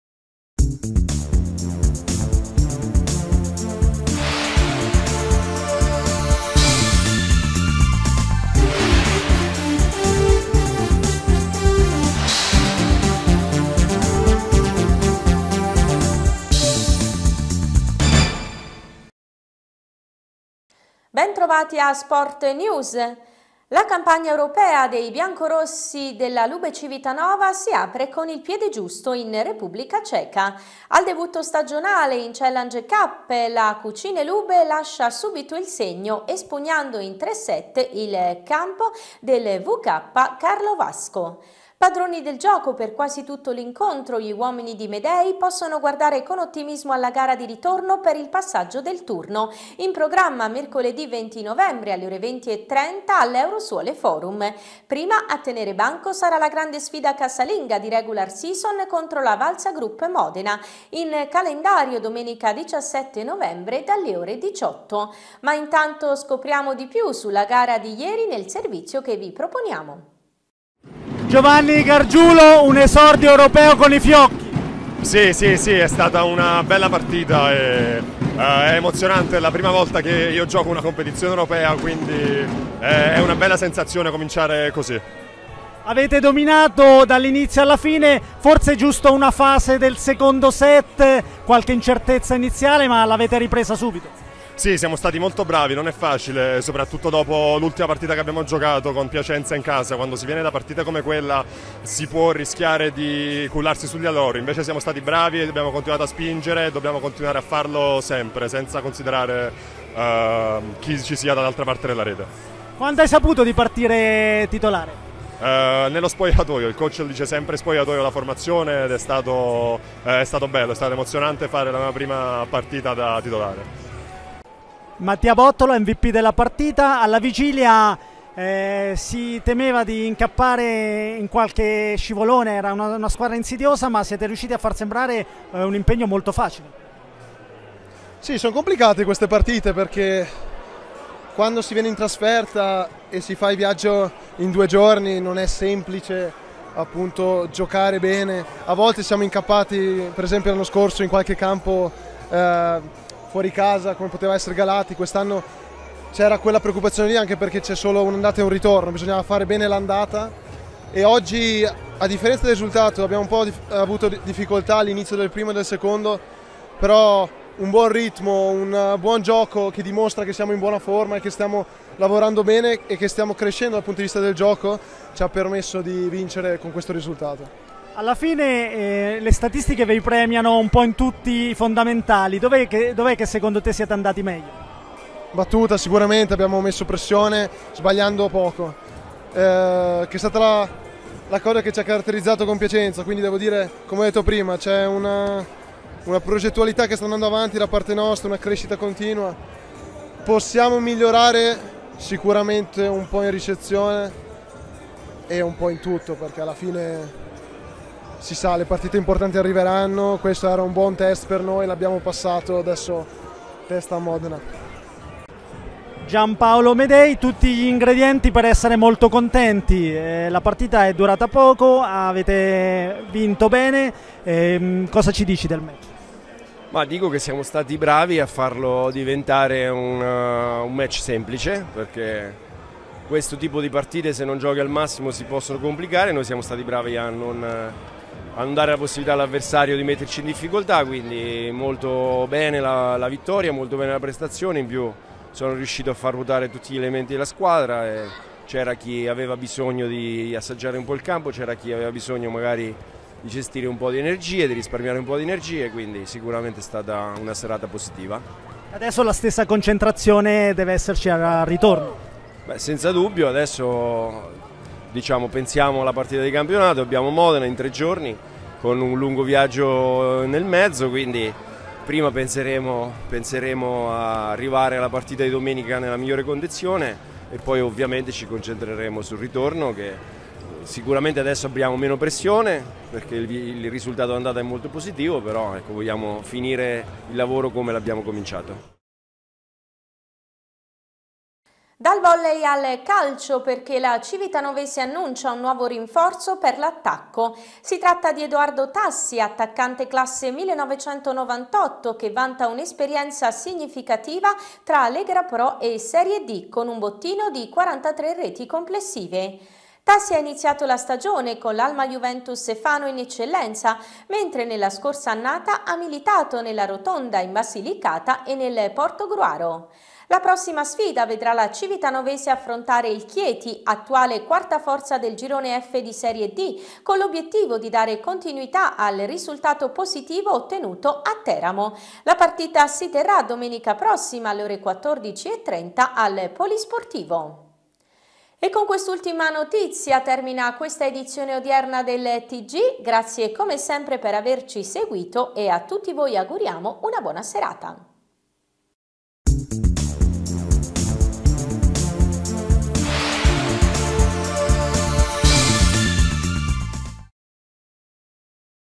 Sport News 15 novembre 2024
Le notizie sportive dalle Marche a cura di Canale 16